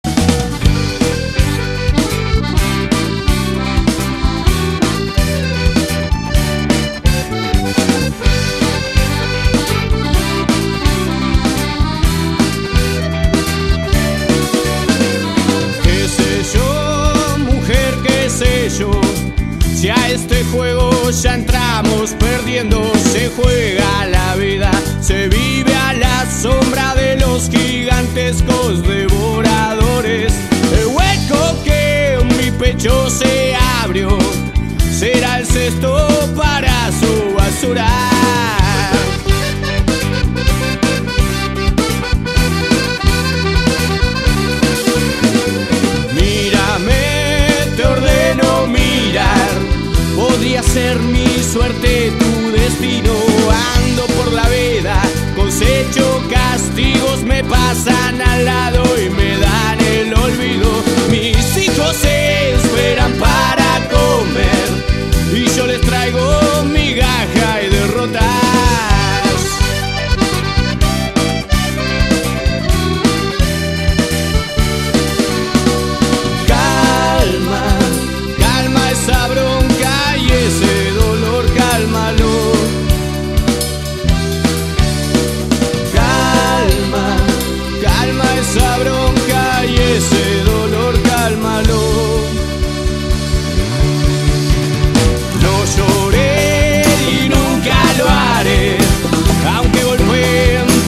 Goles 1º ronda play off vuelta TDI.mpg
Goles de la 1º ronda de play off TDI - Partidos de vuelta
Gentileza TR El Deportivo